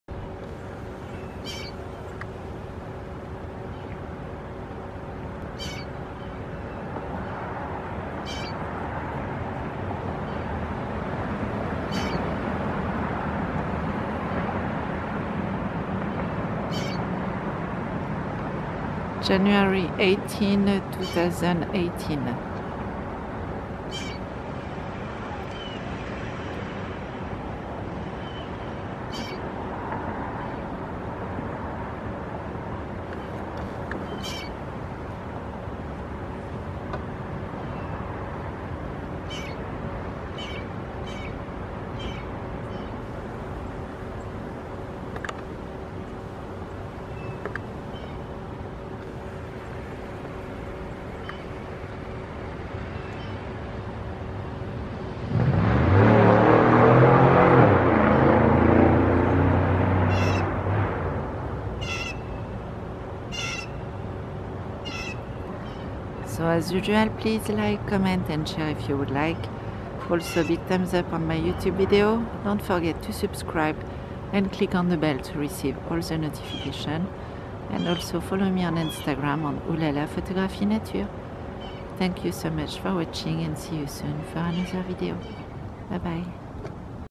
The Woodlands TX Bald Eagles - Martha & George W and their Twins Eaglets Miracle & Hope - 01.18.2018
George was sat at the tree near to the nest for a while then flew away. During that time, Martha was feeding his 21 days old Eaglets Miracle & Hope. I don't know if it's because of the cold day, but today they went back and forth with food for their babies : fish and baby possum at the end of the day